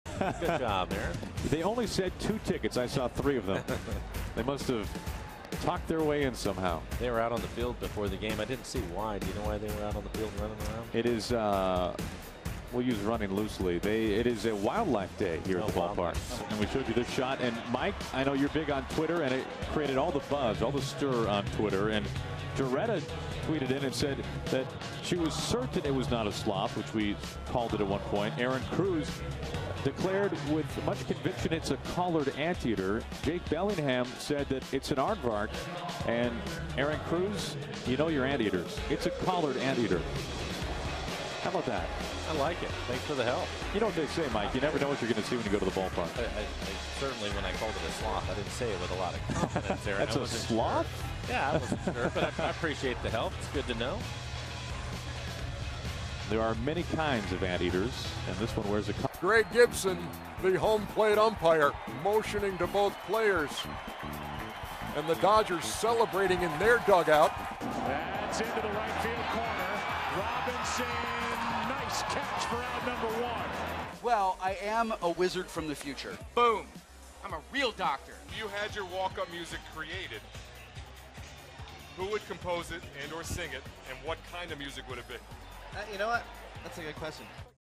Mariners booth has fun with Wildlife Day
4/17/14: The Mariners broadcast booth has some fun with a pair of penguins and an anteater as part of Wildlife Day at the ballpark